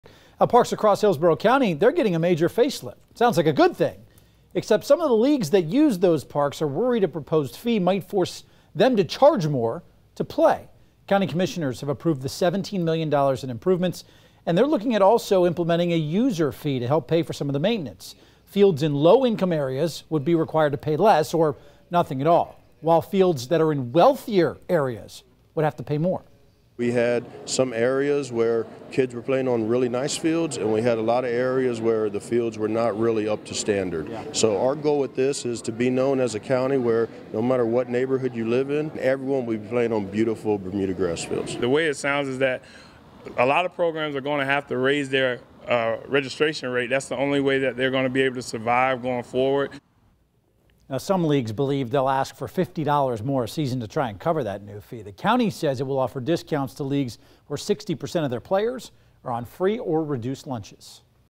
Hillsborough County looks to charge leagues to use fields
Parks across Hillsborough County are getting a major facelift.